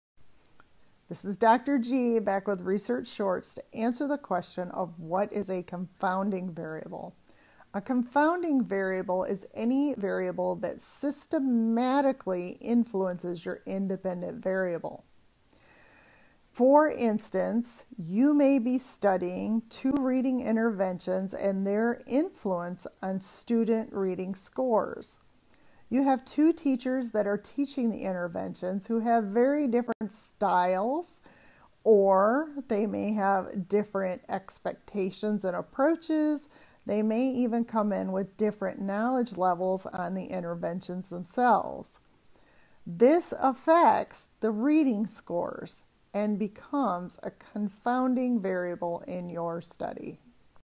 What is a confounding variable?
In this research video, Dr. G answers the question of what is a confounding variable?

Want to say thank you for what I'm doing?